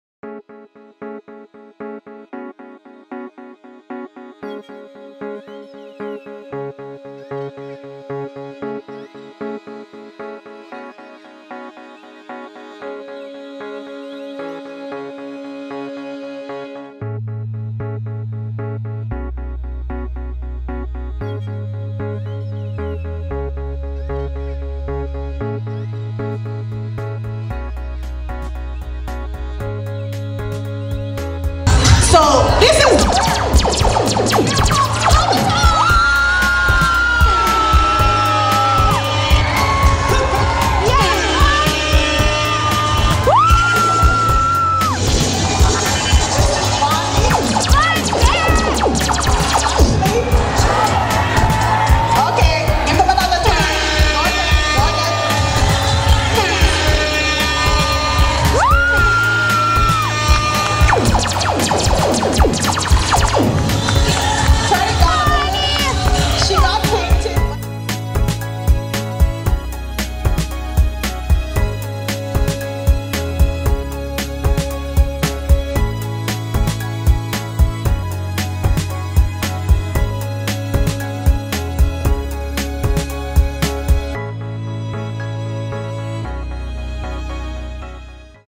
Hair Fashion Runway Model Bodypainted as Avatar Style By Joshua Mayhem
Bodypainted Hair Fashion Runway Model Avatar Style
Natural Hair Show Toronto
By Joshua Mayhem
Concept, style, costume and Hair by Hot Secret
Video by Joshua Mayhem